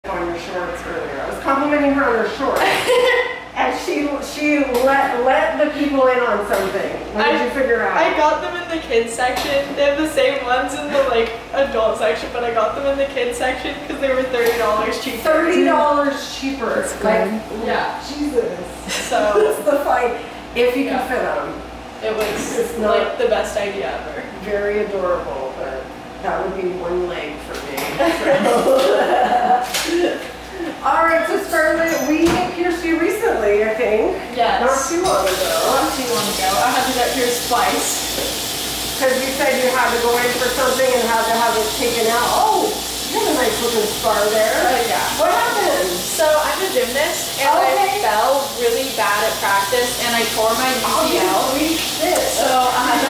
Teaser to Scarlett's New Piercings: Tommy T's Body Piercing Huntington Beach, CA
Daith and Conch Piercings done at Tommy T's Body Piercing in Huntington Beach, California. Jewelry is Implant Grade Titanium Body Jewelry.